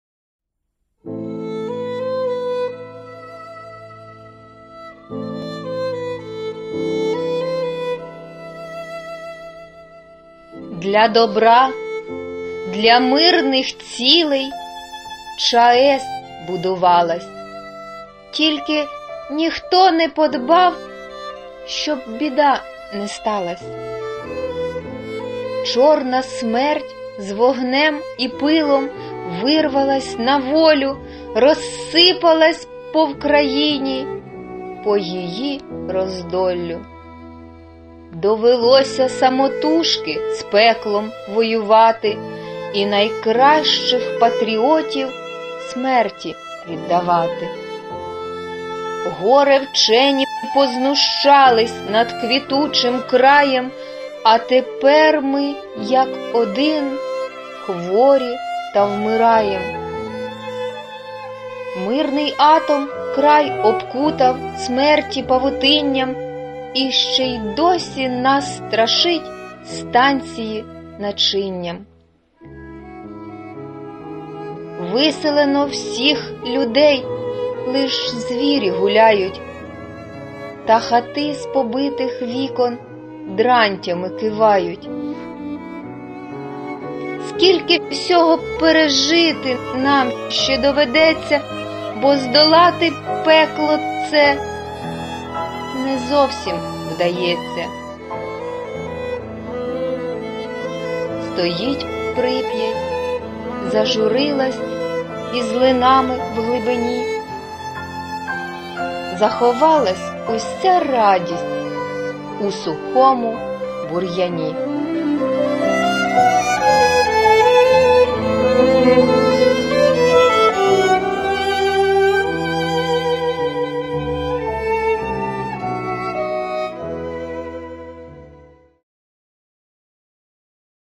ЧОРНОБИЛЬ (автор - Люба Решетнік-Славна)
Вірш Люби Решетнік (Славної) написаний до 30-ї річниці Чорнобильської трагедії.

Вірш читає Феденко О.